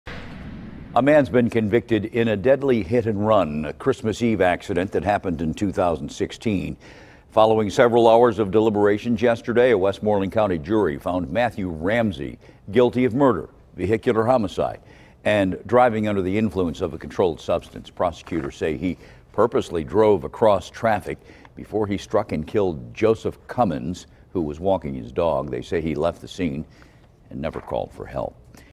Man Convicted In Death Of Westmoreland Co. Man Jogging With Dog
A man has been convicted of third-degree murder in the death of a man struck by a car as he was jogging with his dog in East Huntingdon on Christmas Eve in 2016.